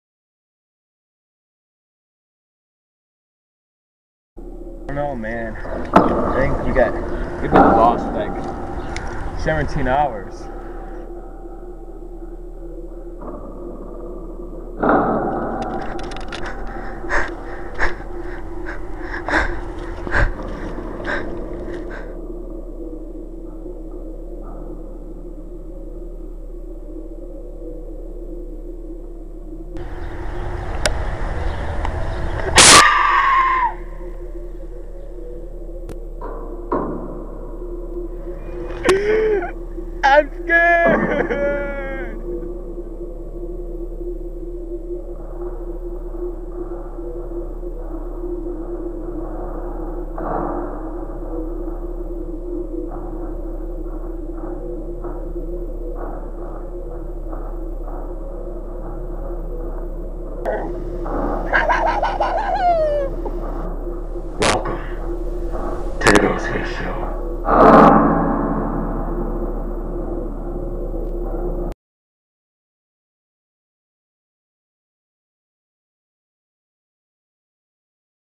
The Ghostface Show, Vol.1 [Trailer] 2010
The official trailer to "The Ghostface Show, Vol. 1". Full of comedy! Look out for the video on Youtube. Starring: Cody Branham, Nolan Bachellor, Austin Mora, Tony Mansfield, Tanner Branham, and Mackenzie Bryant.